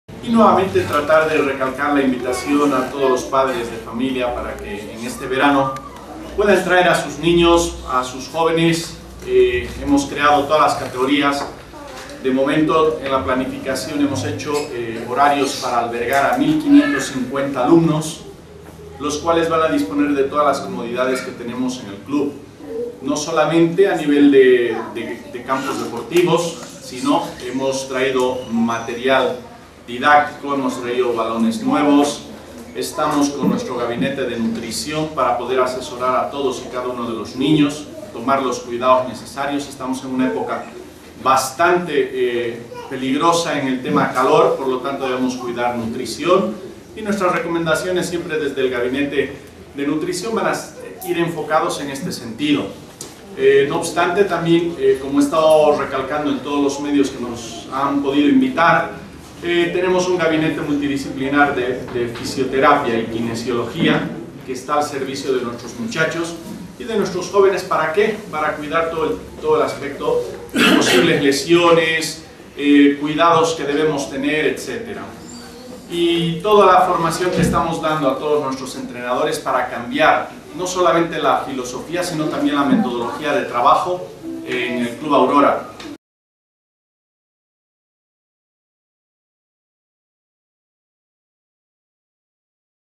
Álvaro Villarroel
El gerente deportivo de Aurora, Álvaro Villarroel, promociona la escuela de verano del celeste.